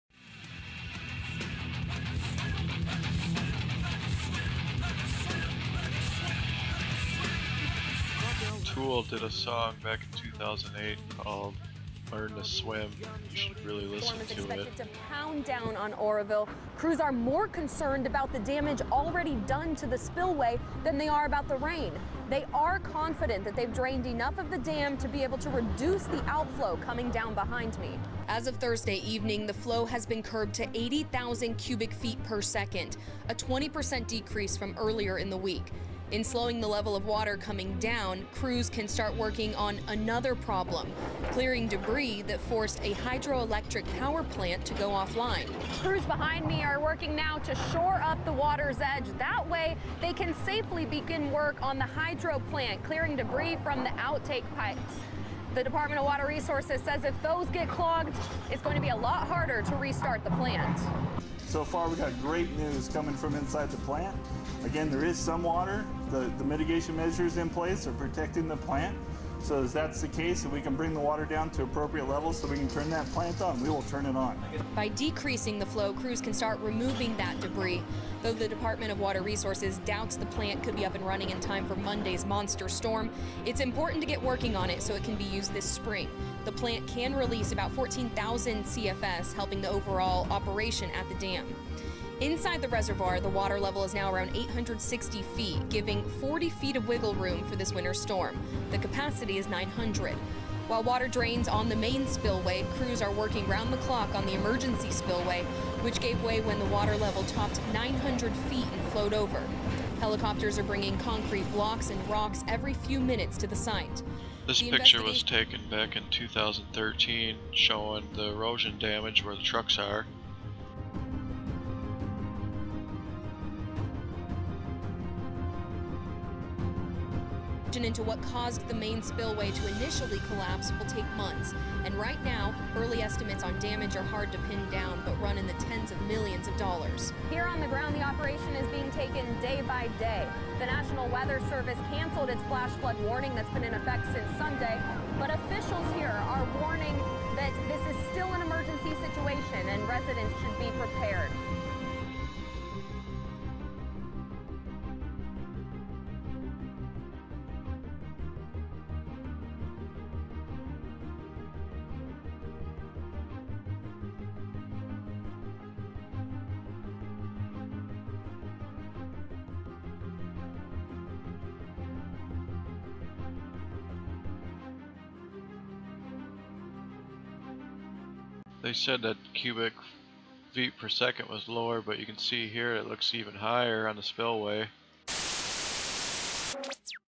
Oroville Dam Repair update & Waterlevel Timeline
report 2-17-17 of the ongoing situation at the Oroville Dam in California. At the end i did a five day photo timeline of the water level within the primary Spillway. I did not see the reduction in CF ran through the spillway in fact i see an increase.